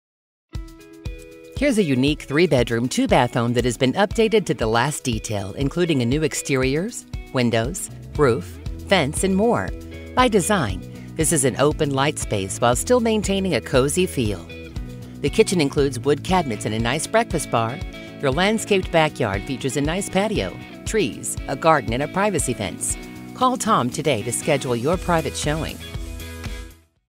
Tom Precella presents 465 E. Sutton Circle Lafayette, CO
465 E. Sutton Circle, Lafayette, CO

This is not just a beautiful home, but mostly everything has been updated or replaced. Such as new Siding, New Windows, Blinds, Newer Roof, Gas Fireplace, New Fence, New Gutters, Hardwood Floors, Sky Lights. By design this is an open light space that makes you feel good. The three beds upstairs are all decent sized with plenty of light. Basement is partially with rec room, and work bench. The back yard is private and a great environment, with Deck, threes, and several gardens.